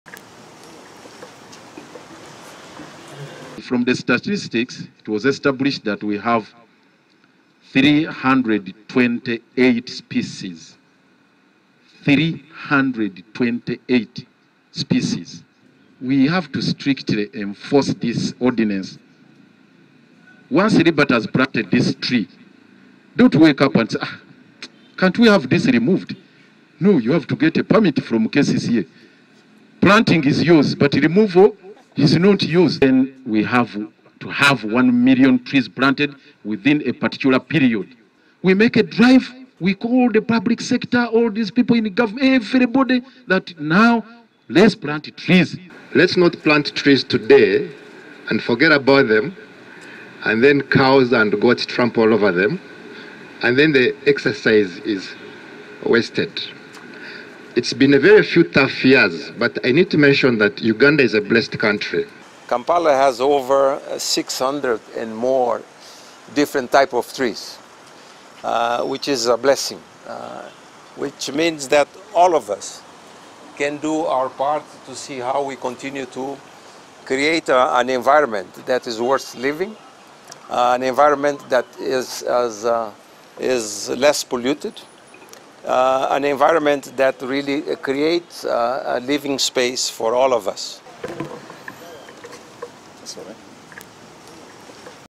KCCA enters partnership to plant over a million trees
Kampala Capital City Authority, KCCA in partnership with My tree plant initiative and Liberty Life assurance have launched a campaign dubbed " Make Kampala City Greener" in which more than one million trees will be planted in the areas around Kampala City.  The initiative is also aimed at remembering the lives that Uganda lost due to COVID-19. The target is to have more than one million trees planted by the end of 2023.